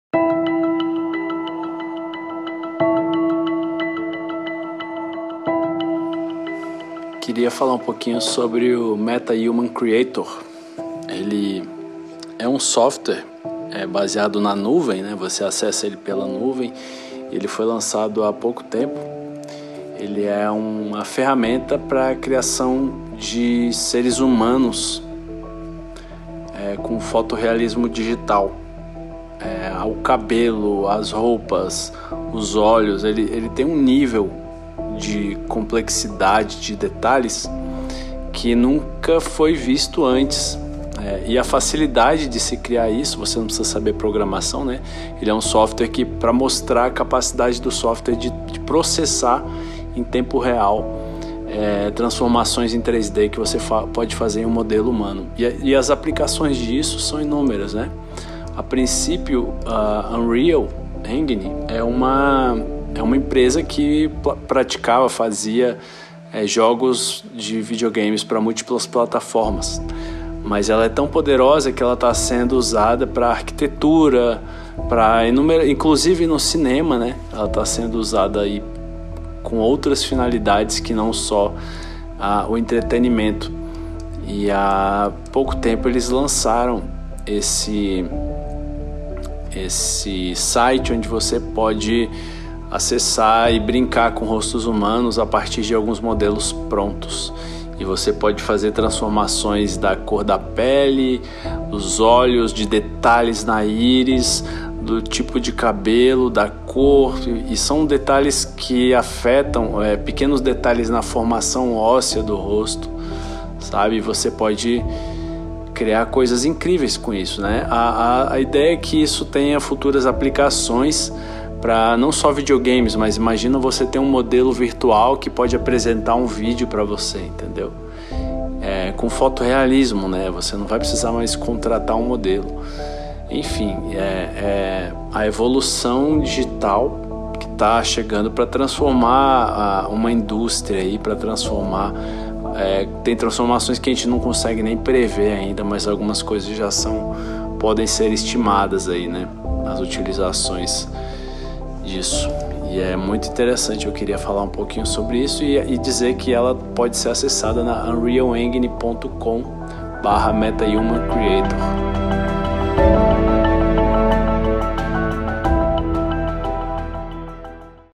Meta Human - Uma ferramenta que cria rostos fotorealísticos num instante
O Meta Human Creator da Unreal engine é uma ferramenta gratuita que está disponibilizada para testes livres no site da Unreal! Ela é capaz de criar seres humanos com foto realismo extremamente poderoso em questão de minutos!

INFO
CINE 81 - Produção de conteúdo audiovisual